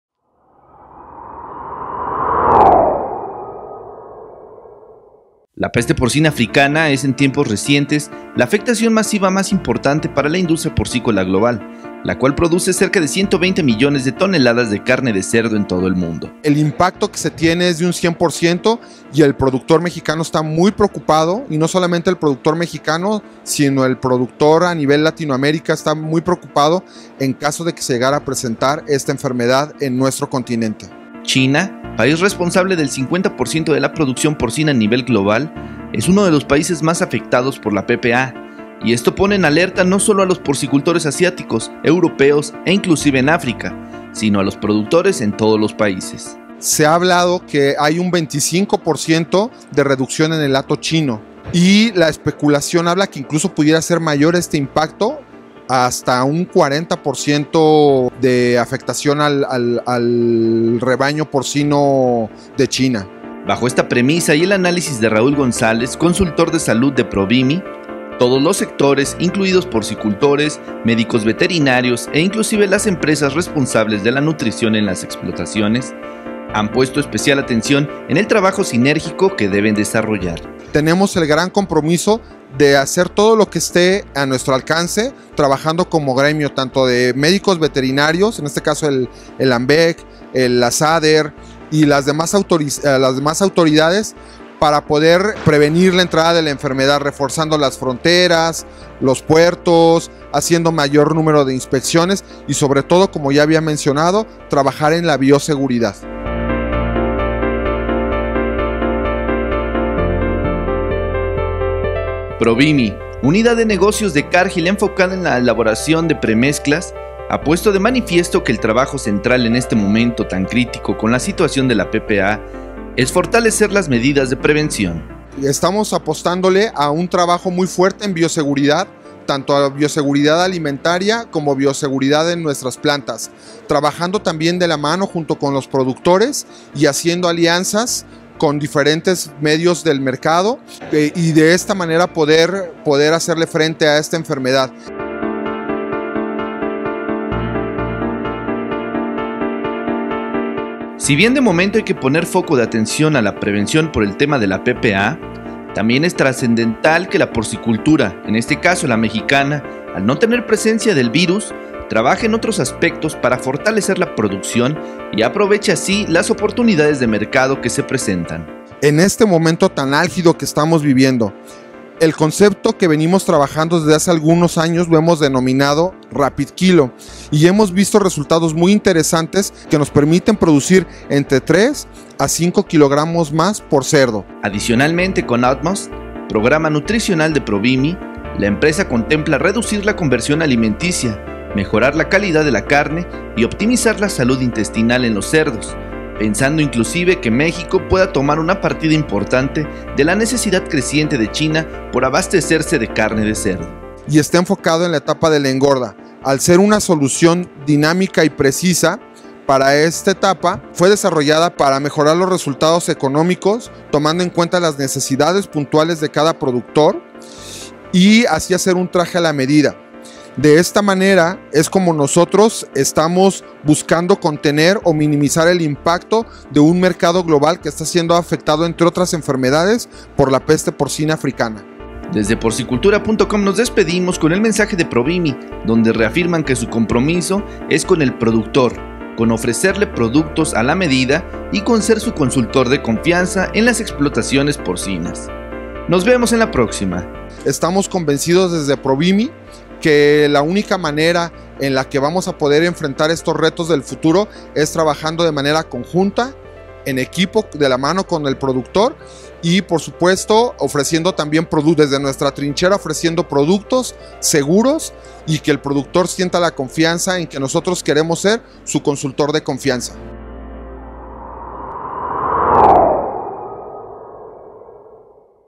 Prevención y aprovechamiento del mercado; recursos para responder a la amenaza de la PPA
Provimi, a través de su unidad de negocio enfocada en la porcicultura, externó su preocupación, pero también su enfoque de trabajo, respecto al riesgo virtual que provoca en México, la entrada de la Peste Porcina Africana (PPA).

Platicamos con el MVZ. Raúl González Martínez, consultor en salud LATAM Norte de la empresa perteneciente a Cargill, quien nos detalló cómo esta afectación, que ha puesto a la porcicultura no solo de Asia, sino de todo el mundo en alerta, también ha hecho que tanto médicos veterinarios, nutricionistas, investigadores y productores, comiencen a trabajar en conjunto para definir líneas preventivas con el objetivo de minimizar al máximo, el riesgo de que este virus ingrese a nuestro país.